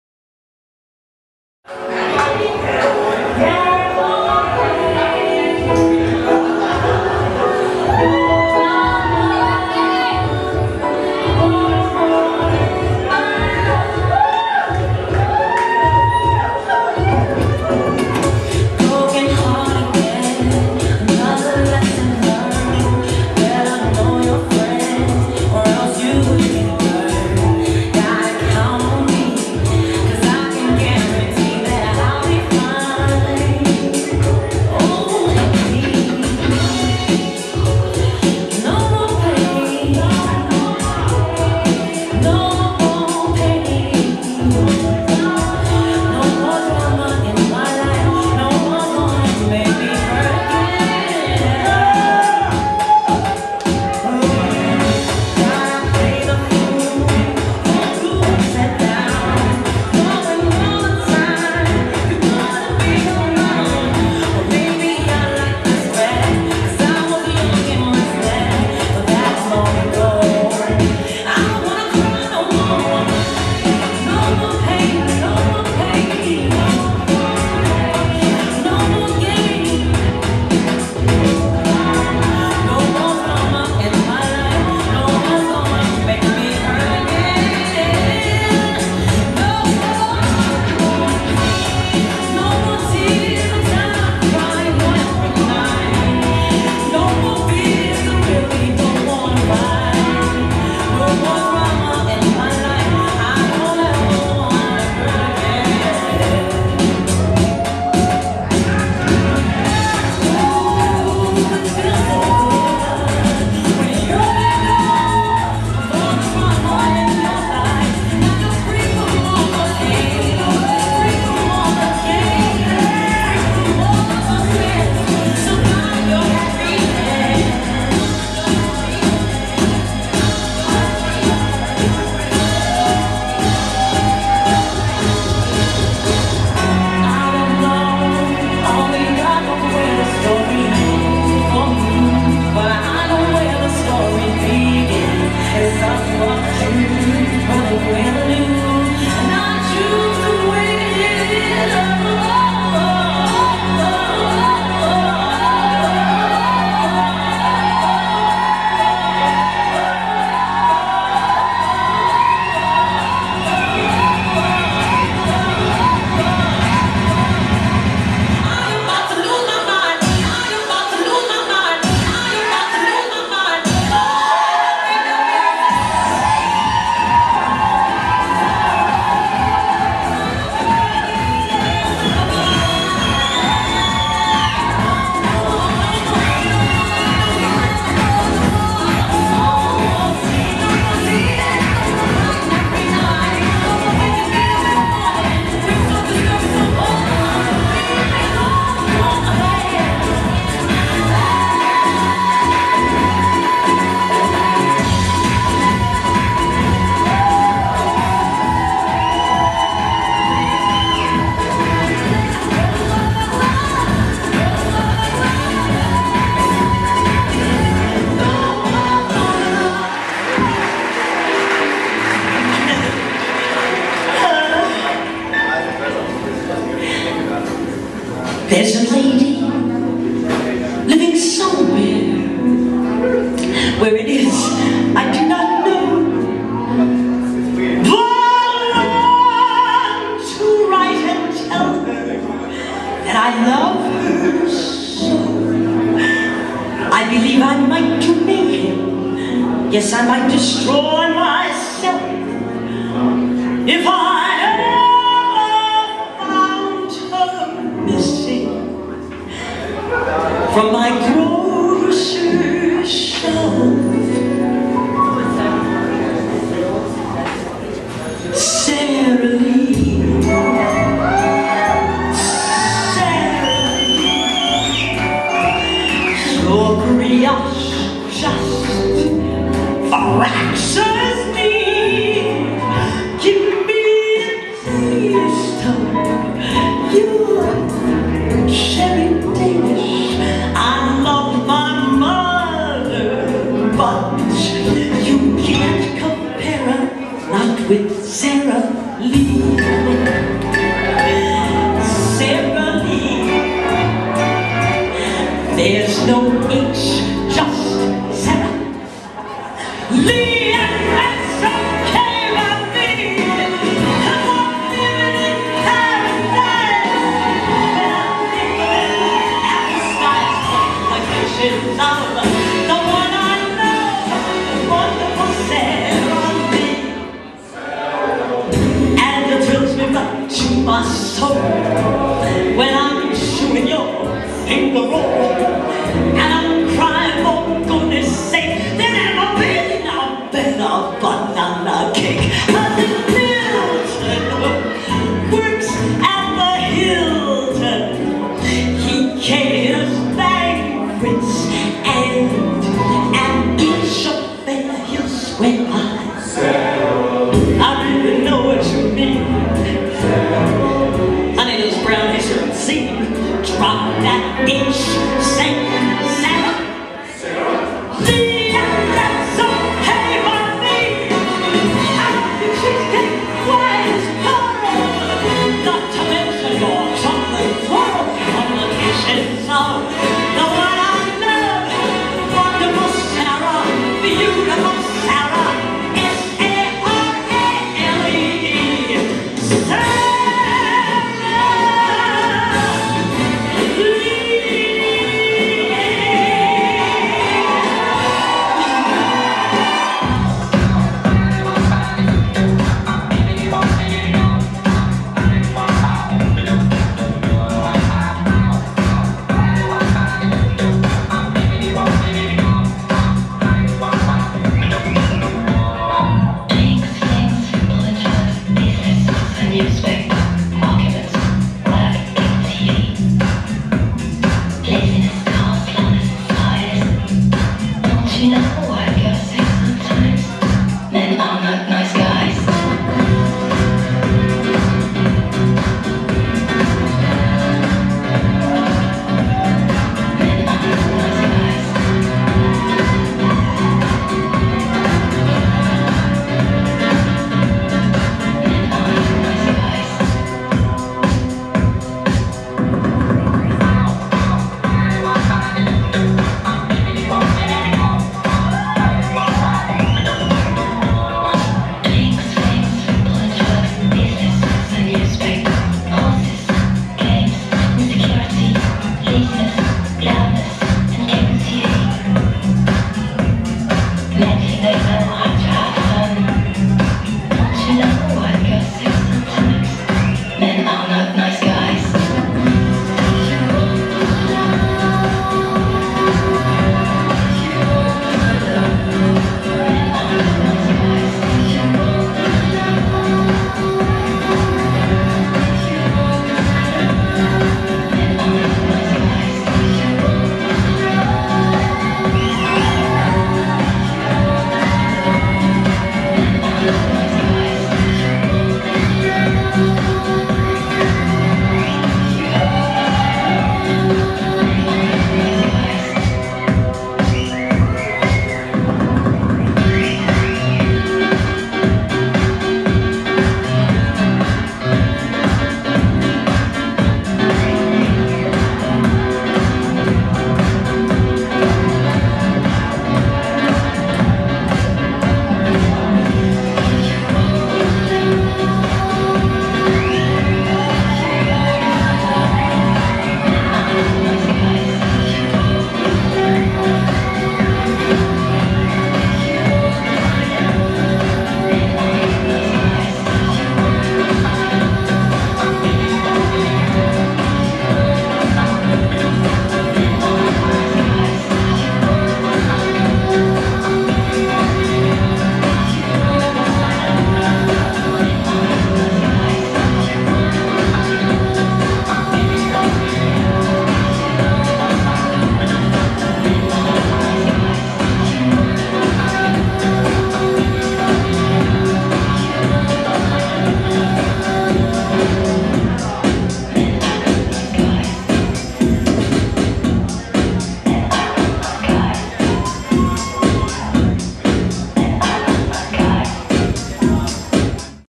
Queens For Queensland Fundraiser Part 4
On A Hot Sunday Night, Adelaides Finest Turned Out, To Show Our Support For Our Queensland Friends Affected By The Floods!
Videography By Nate Rudd :)